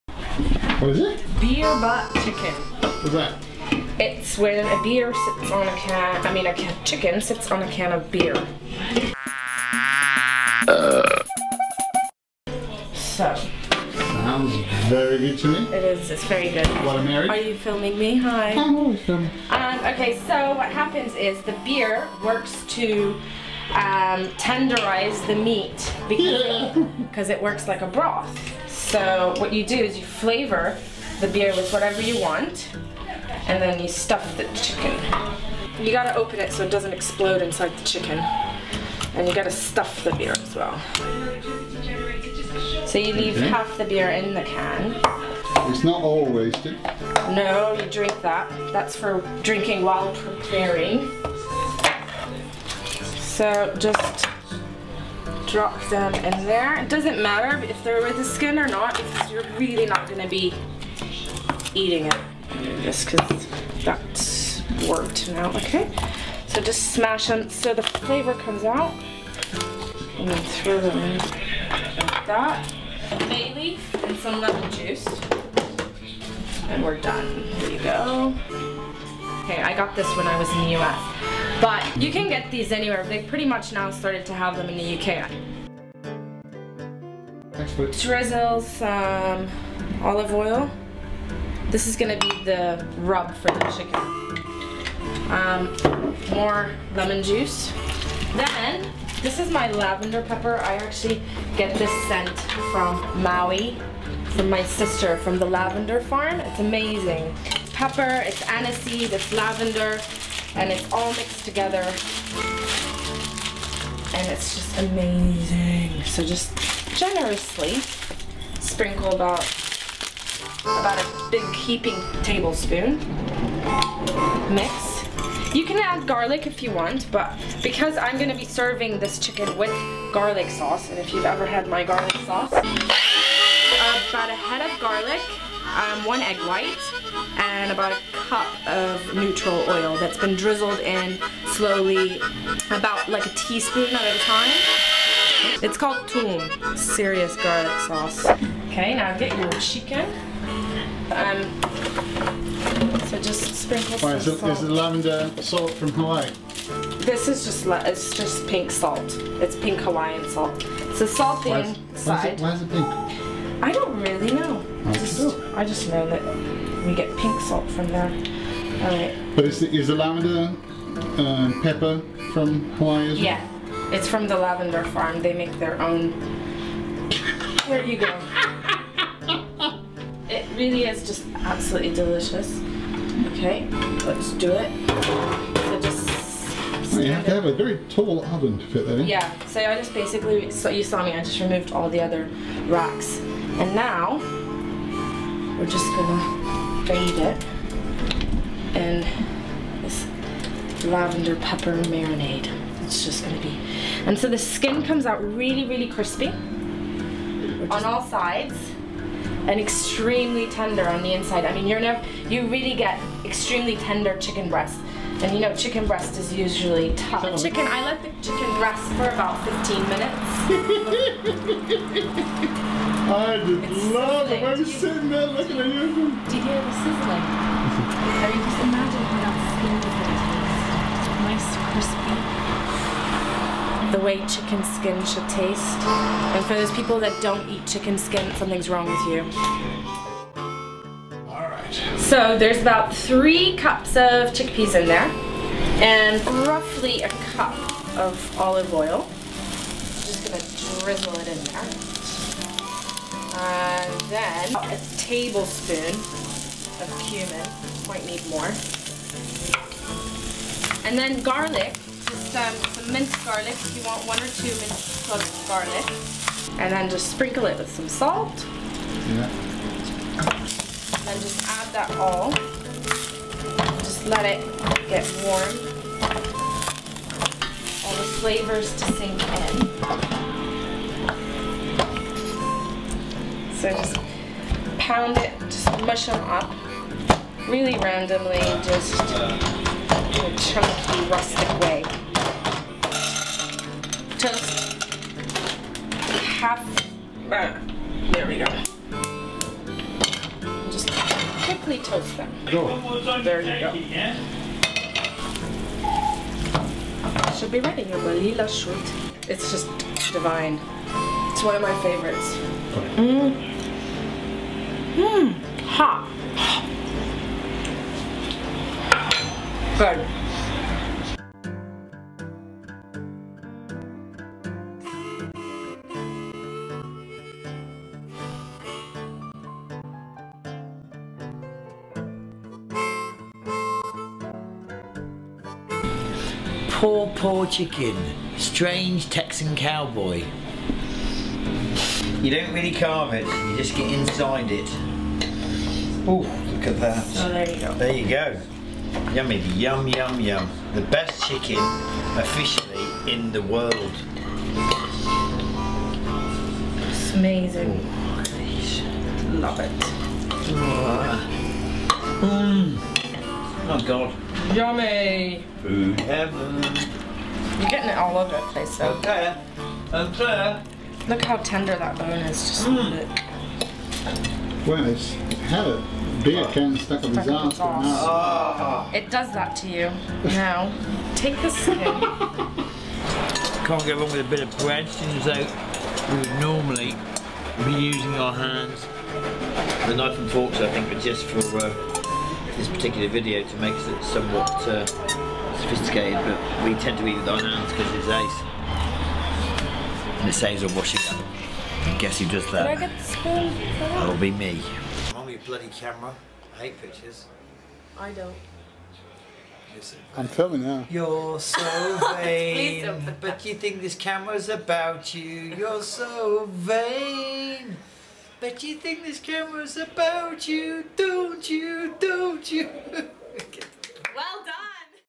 How to make 'Beer Butt Chicken' and Hummus Balila at Bethany's
- Classic Brazilian food and drinks

I met Bethany and Chris at the convention in London. They invited me over to their place in Brighton to watch some of the World Cup games, have a beer and cook together. So I brought the camera to capture a few shots for CucaBrazuca.com.